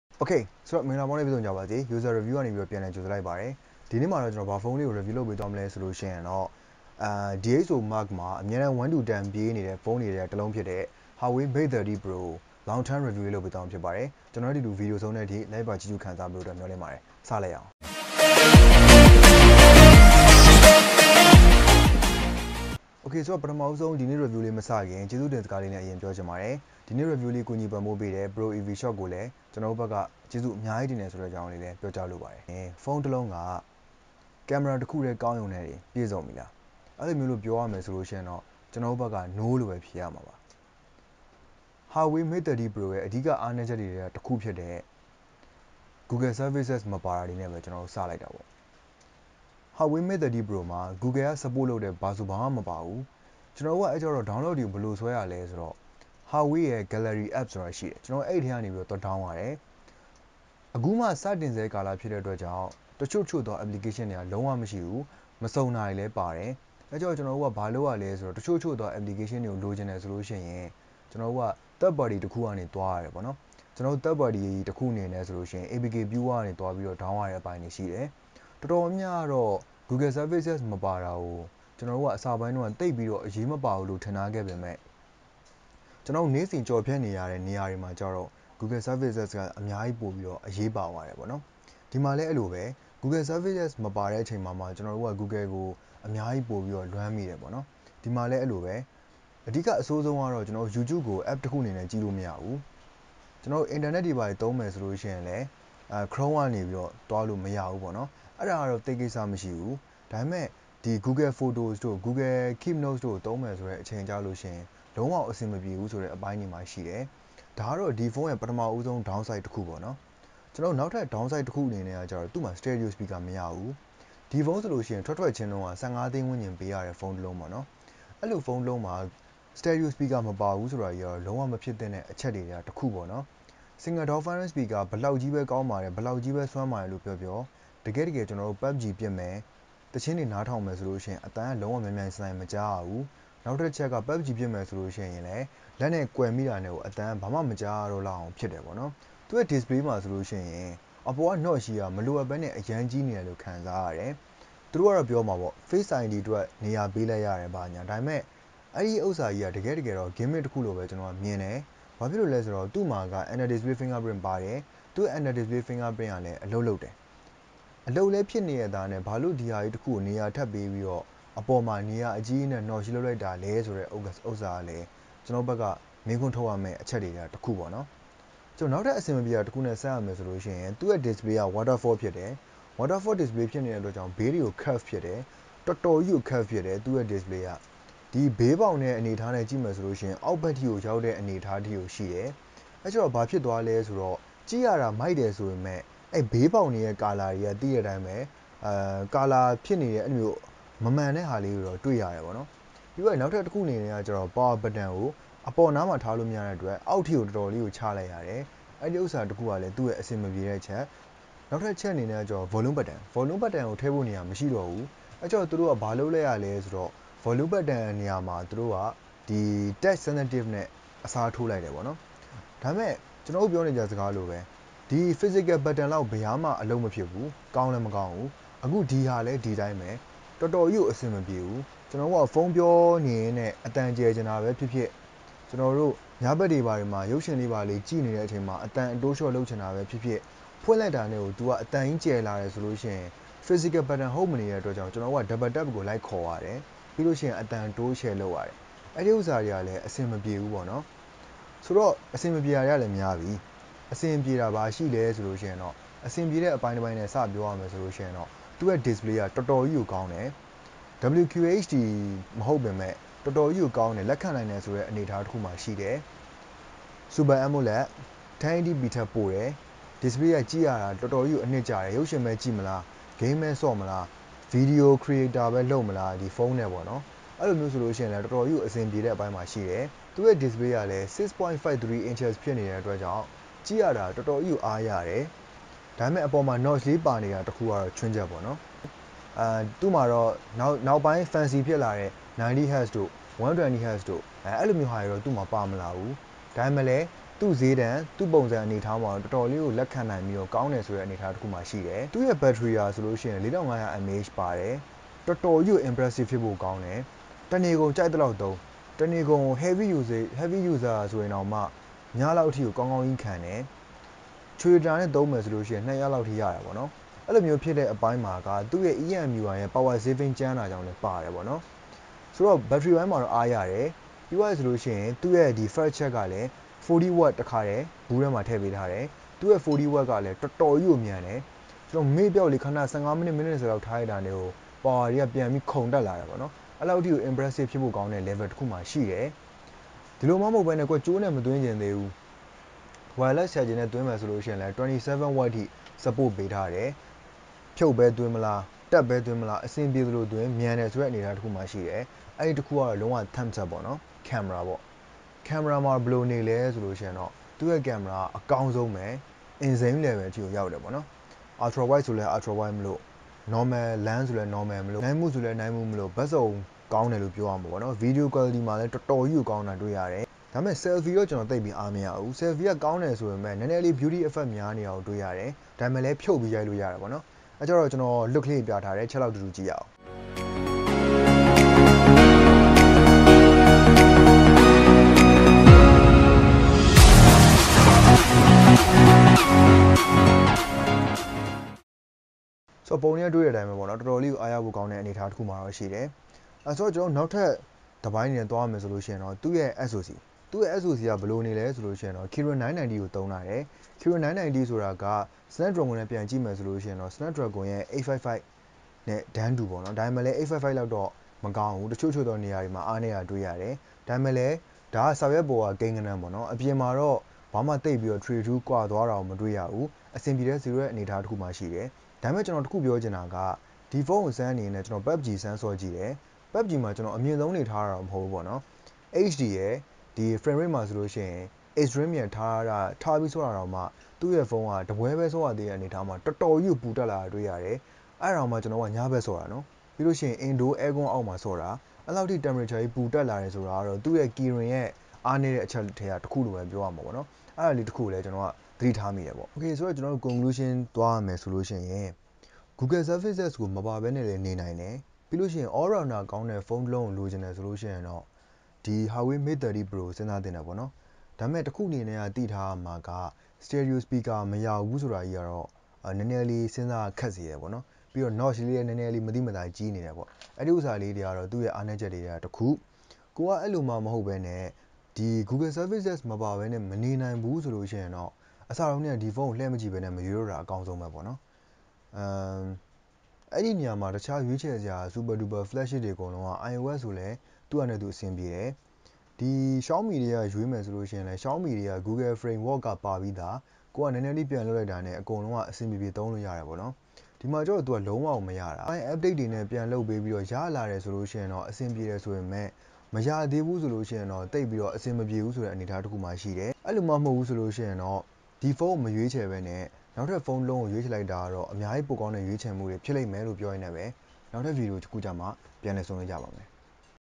Huawei Mate 30 Pro Full Review
Can survive without Google?
Best Camera Phone Before P40